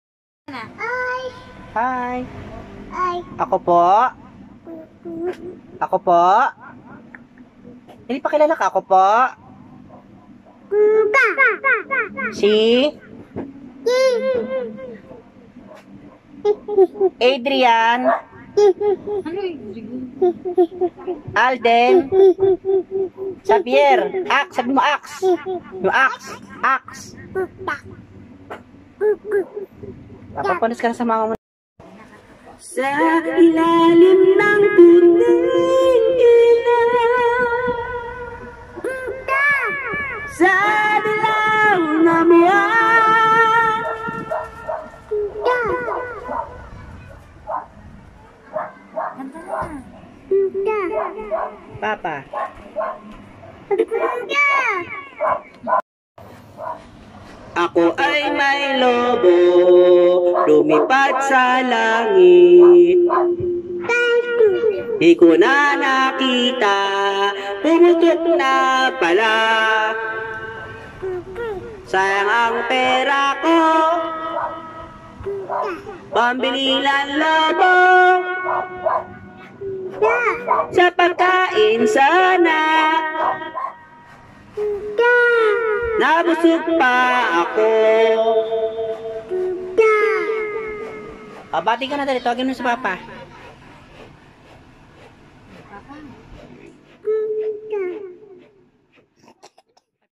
TawagNgTanghalanKampiyon Batang Singer naagaw ang Golden Mikropono
Ang makulit at bibo Kong APO sa pamangkin hindi nagpatalo sa kantahan kaya siya ang kasalukuyang nakakuha Ng Golden Mikropono.

Kumakanta ako Jan tapos bigla siyang lumapit saken inagaw mic kaya duet na Lang kami hahaha.. Love namin si Baby Ax pogi😍